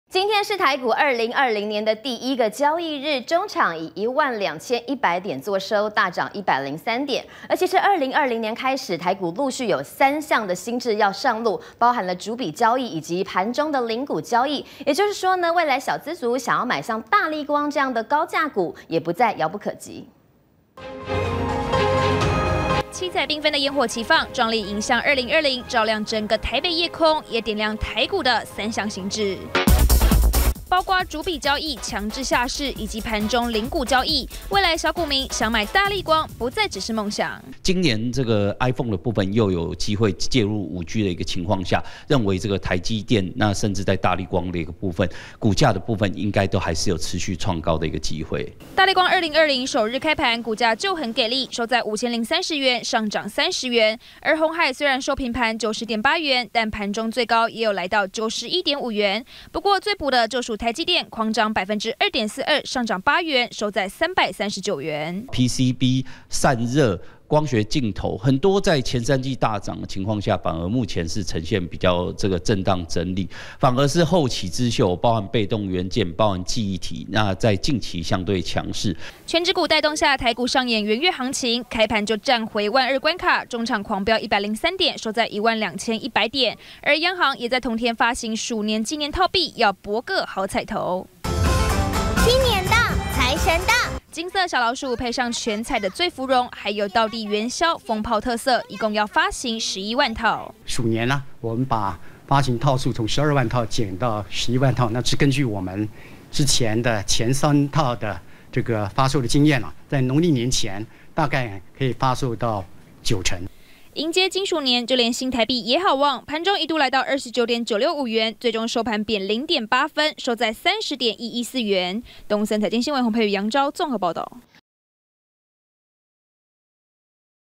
台股2020開紅大漲百點 站穩「萬二」關卡
今(2)日是台股2020年第一個交易日，終場以12100點作收，大漲103點，而其實2020開始台股陸續有3項新制要上路，包含逐筆交易以及盤中零股交易，也就是說未來小資族，想買大立光這樣的「高價股」，不再遙不可及！